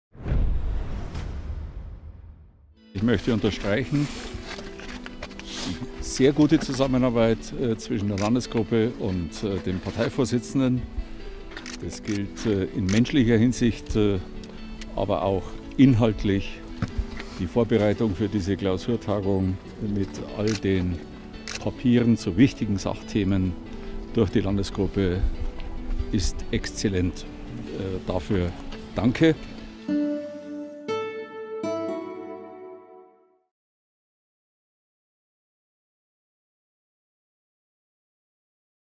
Seehofer dankt der CSU-Landesgruppe
Vor Beginn der Klausurtagung dankt Horst Seehofer der CSU-Landesgruppe für die gute Zusammenarbeit und die inhaltliche Vorbereitung der Klausurtagung in Wildbad Kreuth.